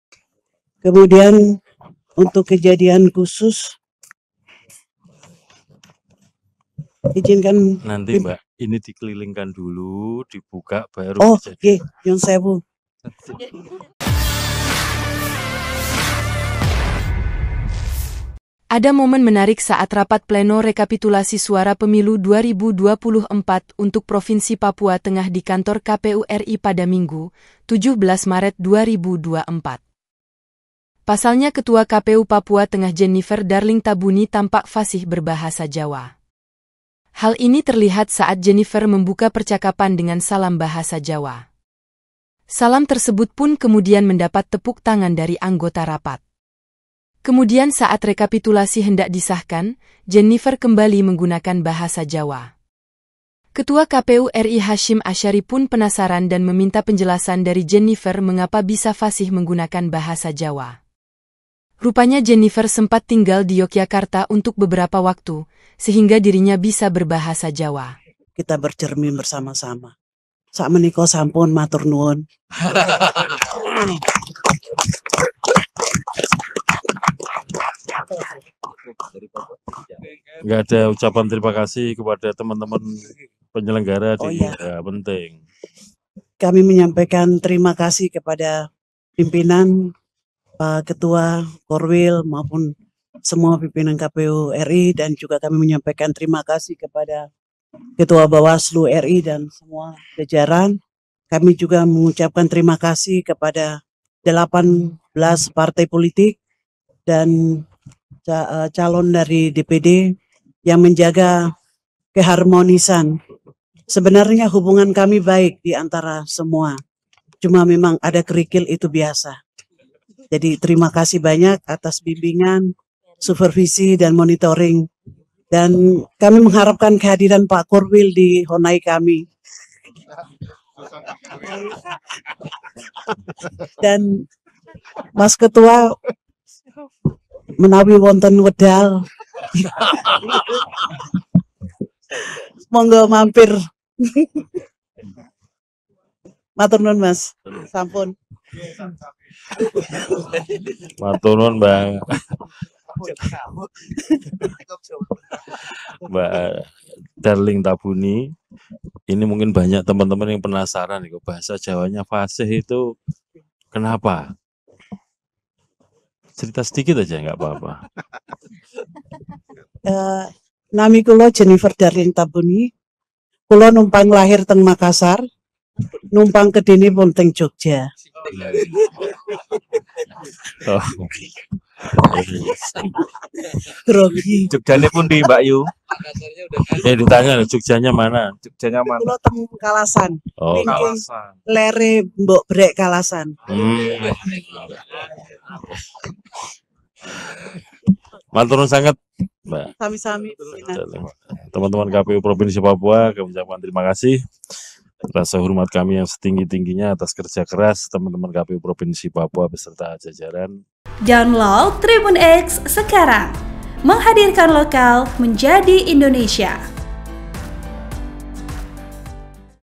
Momen Ketua KPU Papua Tengah Fasih Berbahasa Jawa saat Rekapitulasi Suara Pemilu 2024
Ada momen menarik saat rapat pleno rekapitulasi suara Pemilu 2024 untuk Provinsi Papua Tengah di Kantor KPU RI pada Minggu (17/3/2024).

Pasalnya Ketua KPU Papua Tengah Jennifer Darling Tabuni tampak fasih berbahasa Jawa.

Hal ini terlihat saat Jeniffer membuka percakapan dengan salam bahasa Jawa.